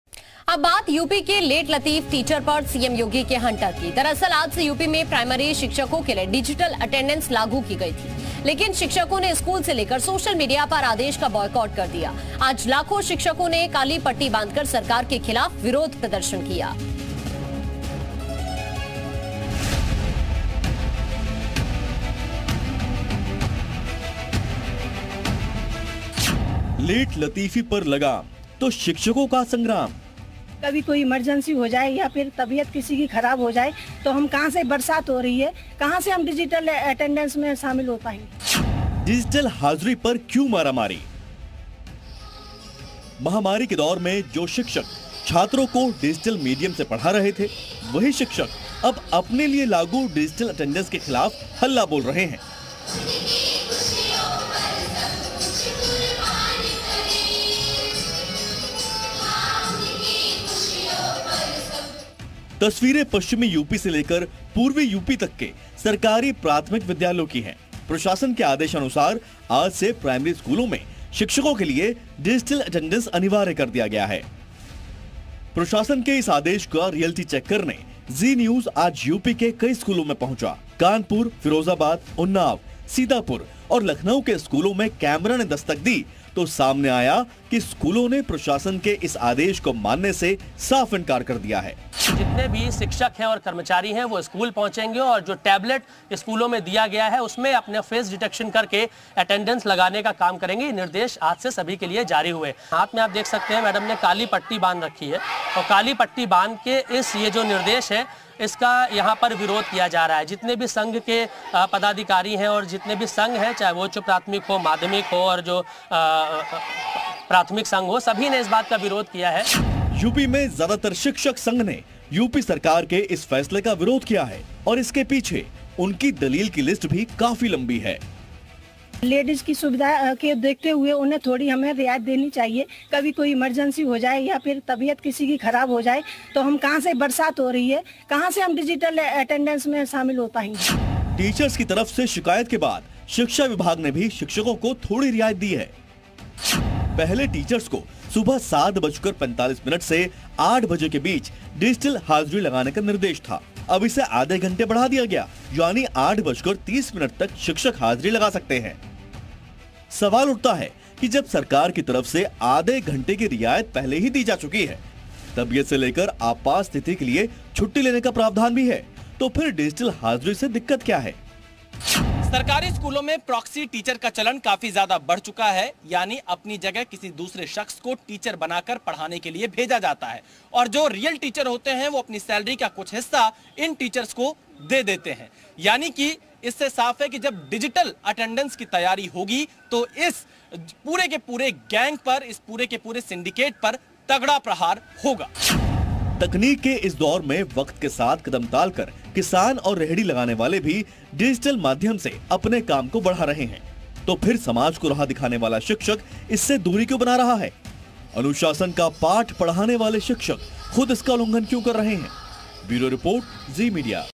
Digital Attendance से क्यों डरे गुरुजी ? देरी पर 'लगाम' तो शिक्षकों का संग्राम? | UP Teacher Protest
Khabar Uttar Pradesh: डिजिटल हाजिरी से क्यों डरे गुरुजी ? देरी पर 'लगाम' तो शिक्षकों का संग्राम? लेट-लतीफ टीचर..योगी का 'हंटर'

Lucknow: Digital attendance of teachers from today, resentment among teachers of council schools, teachers protesting against digital attendance, protest by wearing black bands till 14th July.